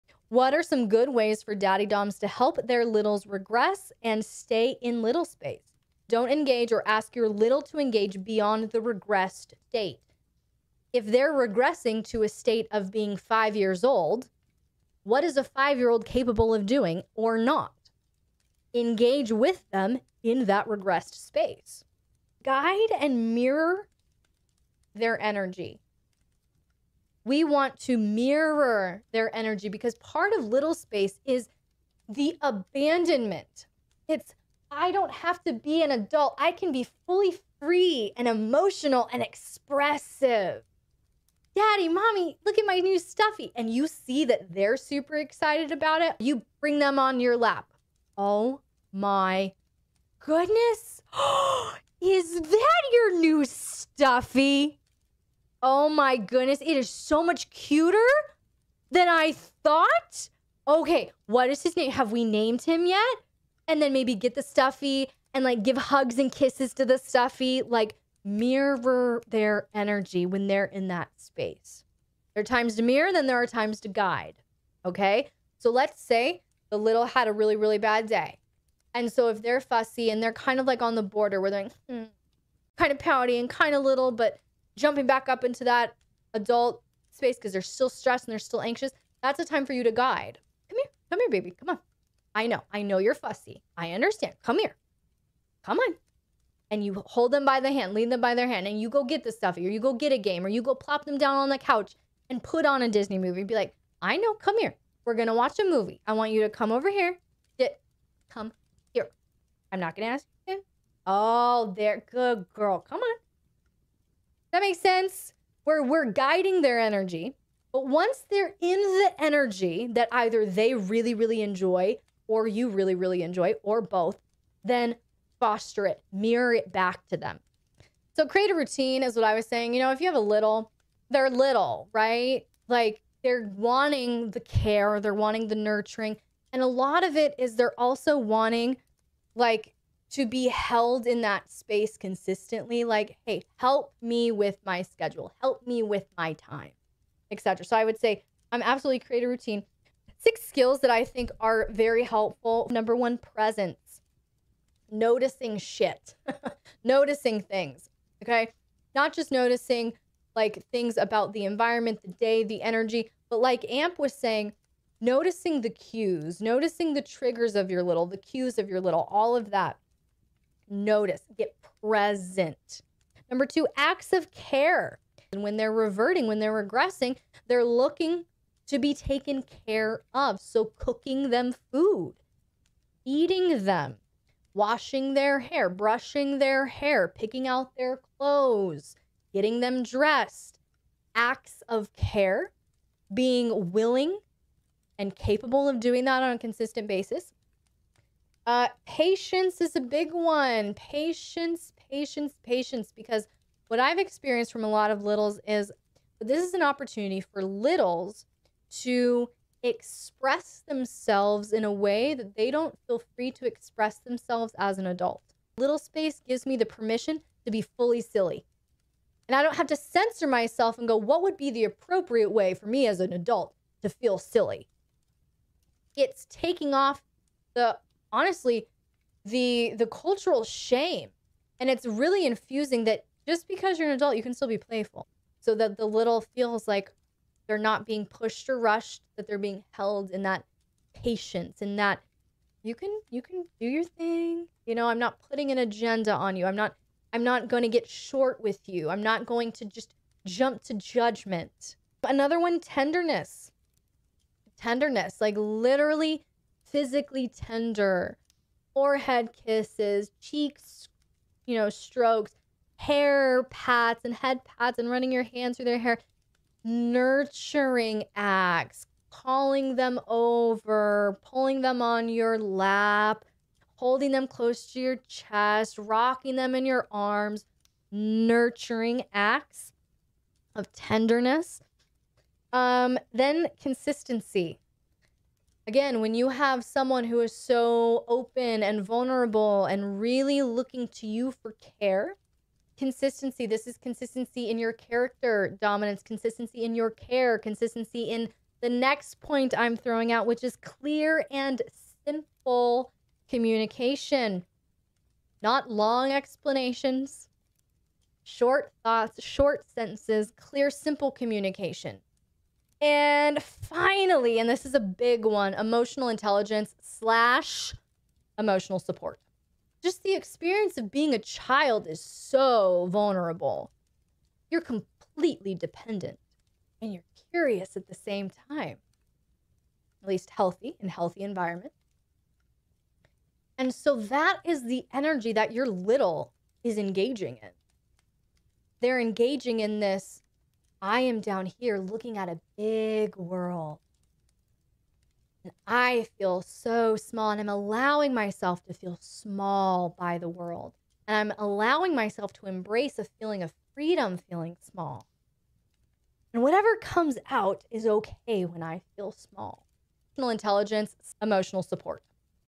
Ms Elle X Working with Littles & Little Space (Inner children)
excerpt from Ask Ms. Elle LIVE! Ep. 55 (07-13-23)

transcript:
what are some good ways for Daddy Doms to help their Littles regress and stay in little space?

- don't engage or ask your little to engage beyond the regressed state
if they're regressing to a state of being five years old
what is a five-year-old capable of doing, or not?
engage with them in that regressed space

- guide and mirror their energy
we want to mirror their energy
because part of little space is the abandonment
it's I don't have to be an adult I can be fully free and emotional and expressive
daddy mommy look at my new stuffy and you see that they're super excited about it
you bring them on your lap
oh my goodness oh is that your new stuffy
oh my goodness it is so much cuter than I thought
okay what is his name have we named him yet
and then maybe get the stuffy and like give hugs and kisses to the stuffy
like mirror their energy when they're in that space
there are times the mirror
then there are times to guide okay
so let's say the little had a really really bad day
and so if they're fussy and they're kind of like on the border with them kind of pouty and kind of little,
but jumping back up into that Adult Space because there's still stressed and they're still anxious
that's a time for you to guide
come here come here baby come on I know I know you're fussy I understand come here come on
and you hold them by the hand lead them by their hand
and you go get the stuffy or you go get a game or you go plop them down on the couch and put on a Disney movie
be like I know come here
we're gonna watch a movie
I want you to come over here
get come here
I'm not gonna ask you again
oh they're good girl come on
that makes sense
we're we're guiding their energy
but once they're in the energy that either they really really enjoy or you really really enjoy or both
then Foster it mirror it back to them

-  Create a routine
if you have a little,
they're wanting the care they're wanting the nurturing and a lot of it is they're also wanting like to be held in that space consistently.
like hey help me with my schedule help me with my time Etc
Absolutely create a routine

6 skills that I think are very helpful

- number one presence, noticing shit noticing things okay not just noticing like things about the environment the day the energy
but like amp was saying
noticing the cues noticing the triggers of your little
the cues of your little all of that
notice get present

- number two acts of care
and when they're reverting when they're regressing
they're looking to be taken care of
so cooking them food feeding them washing their hair brushing their hair picking out their clothes getting them dressed
acts of care
being willing and capable of doing that on a consistent basis

- patience is a big one patience patience patience
because what I've experienced from a lot of Littles is
this is an opportunity for Littles to express themselves in a way that they don't feel free to express themselves as an adult
little space gives me the permission to be fully silly
and I don't have to censor myself and go what would be the appropriate way for me as an adult to feel silly
it's taking off the honestly the cultural shame
and it's really infusing that just because you're an adult you can still be playful
so that the little feels like they're not being pushed or rushed that they're being held in that patience
and that you can you can do your thing you know
I'm not putting an agenda on you I'm not
I'm not gonna get short with you
I'm not going to just jump to judgment

- another one tenderness
tenderness like literally physically tender forehead kisses cheeks you know Strokes hair Pats and head pats and running your hands through their hair
nurturing acts
calling them over pulling them on your lap
holding them close to your chest
rocking them in your arms
nurturing Acts of tenderness

- then consistency
again when you have someone who is so open and vulnerable and really looking to you for care
consistency this is consistency in your character dominance
consistency in your care

consistency in the next point I'm throwing out
- which is clear and simple communication
not long explanations
short thoughts short sentences
clear simple communication

and finally and this is a big one
- emotional intelligence slash emotional support
just the experience of being a child is so vulnerable
you're completely dependent and you're curious at the same time
at least healthy in healthy environments
and so that is the energy that your little is engaging in
they're engaging in this I am down here looking at a big world
I feel so small and I'm allowing myself to feel small by the world
and I'm allowing myself to embrace a feeling of Freedom feeling small
and whatever comes out is okay when I feel small
emotional intelligence emotional support